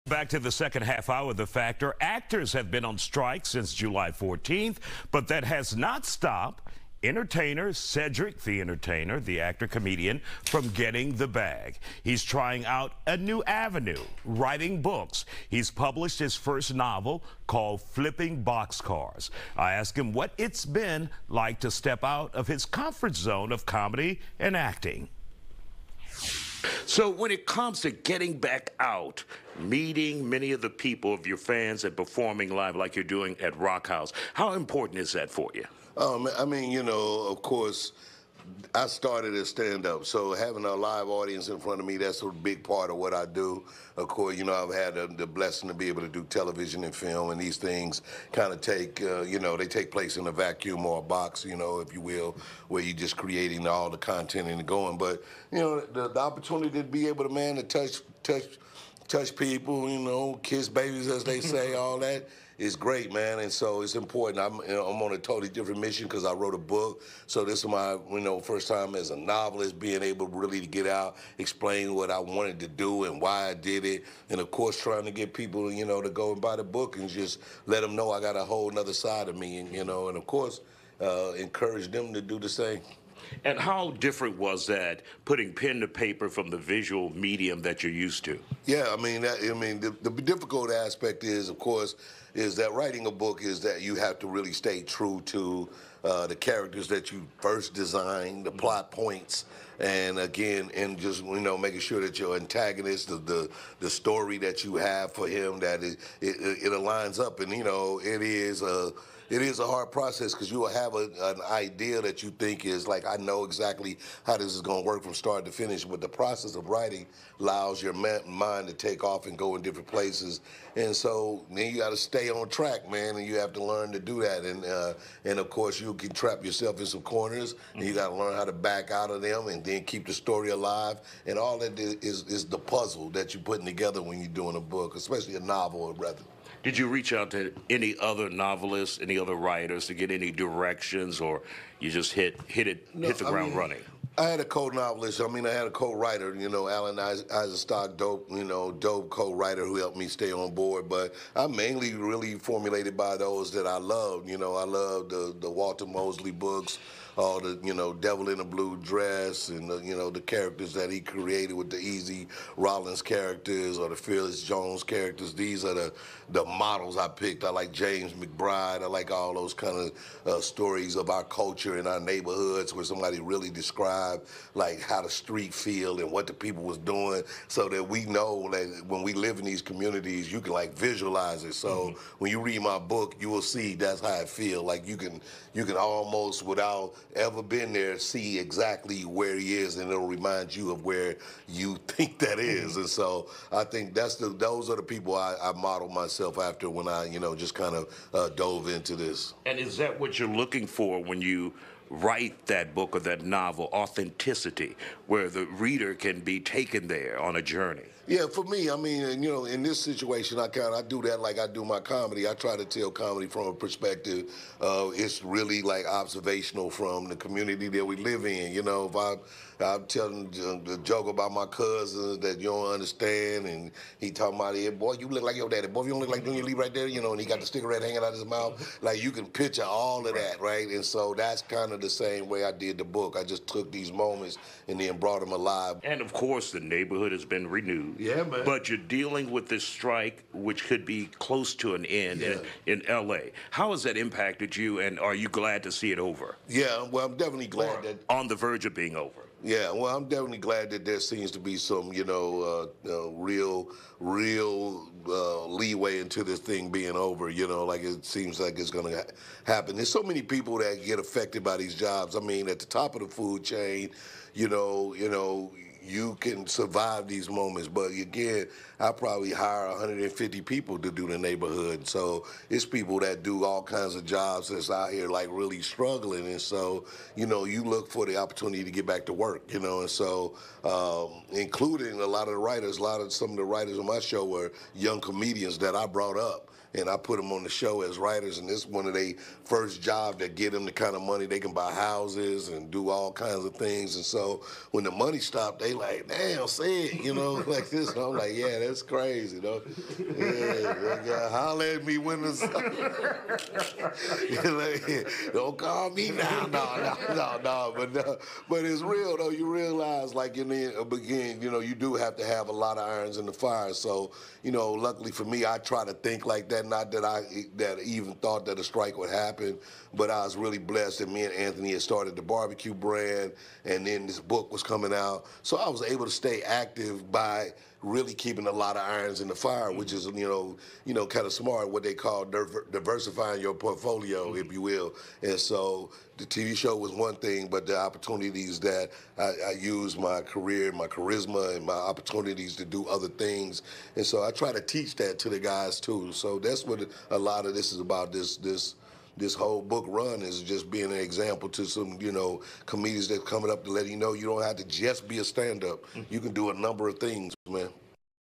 Cedric The Entertainer releases his first novel
Cedric The Entertainer is finding new ways to keep fans interested during the Hollywood strike.